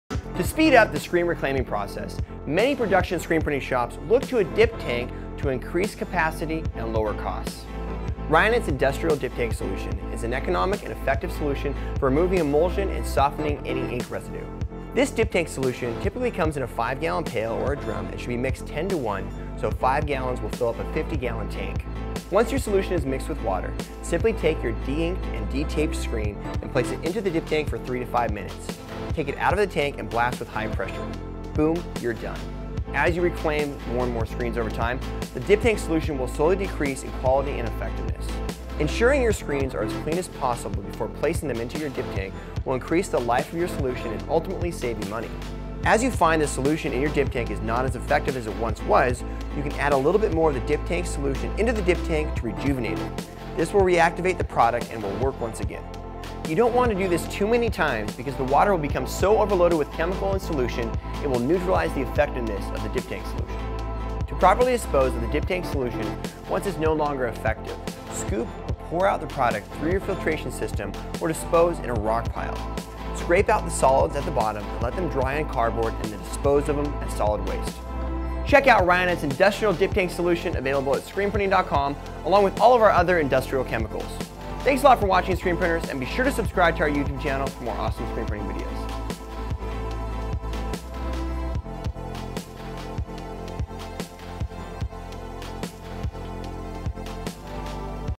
How To Use Ryonet's Industrial Dip Tank Solution for Screen Printing
TRANSCRIPT:
To speed up the reclaiming process, many production screen printing shops look to dip tanks to increase capacity and lower costs.

Ryonet’s industrial dip tank solution is an economic and effective solution for removing emulsion and softening any ink residue.

This dip tank solution typically comes in 5 gallon pail or a drum and should be mixed down 10:1, so one 5 gallon can fill up a 50 gallon tank.

Once your solution is mixed with water simply take your deinked and detaped screen and place it in the dip tank for 3-5 minutes, take out of the tank and blast with high pressure, boom you are done!

As you clean more and more screens over time, the solution in your dip tank will slowly decrease in quality and effectiveness.

Ensuring your screens are as clean as possible before placing them in your dip tank will increase the life of your solution and  ultimately save you money.

If you find that the solution in your tank is not as effective as it once was, you can add a little more dip tank solution into the dip tank to rejuvenate it, this will reactivate the product and it will work again.

Do not do this too many times however because the water becomes so loaded with left over emulsion and ink it neutralizes the effectiveness of the solution.

To properly dispose of the dip tank solution once it is no longer effective, scoop or pour out the product through your filtration system or disposal rock pile. Scrape out the solids at the bottom and let them dry on cardboard, then dispose of it as solid waste.